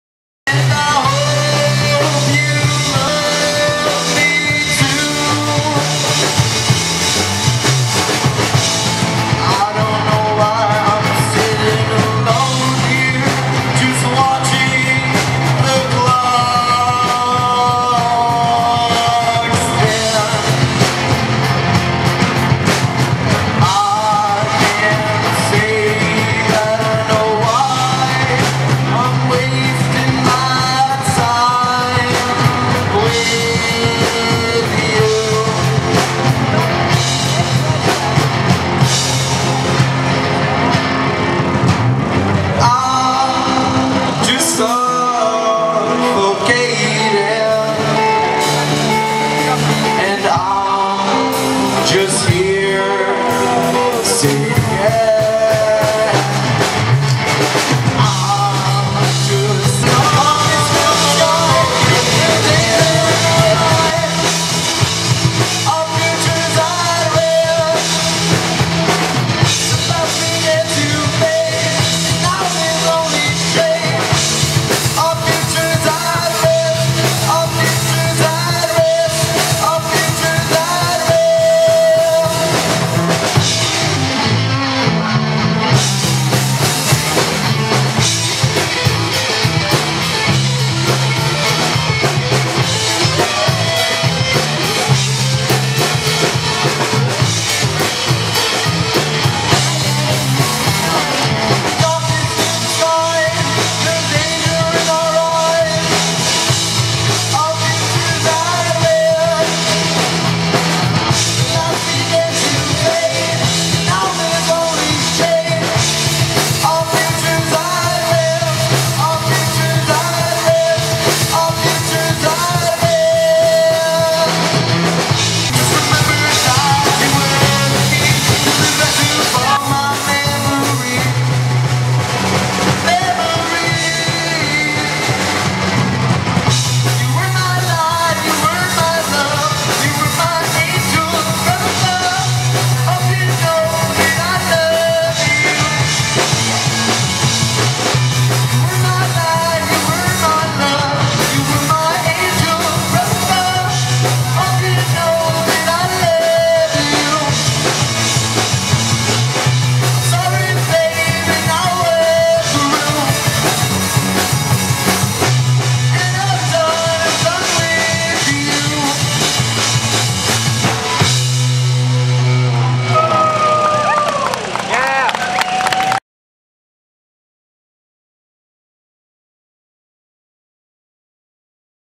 The Riot Police @ Glebe's Got Flavour Ottawa 2010
Three short clips of original songs by a young Ottawa band "The Riot Police" performing at Glebe's Got Flavour on 11 Sept 2010.